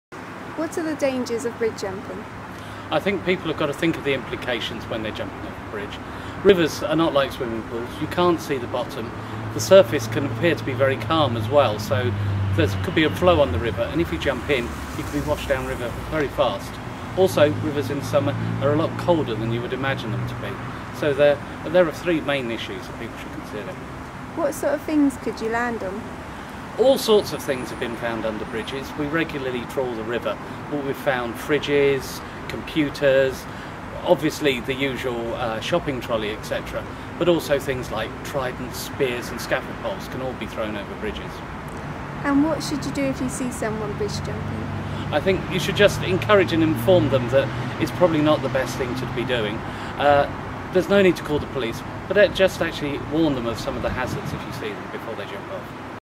Don't jump off bridges - Environment Agency
Our Russell Robson is talking about water safety and the dangers of bridge jumping.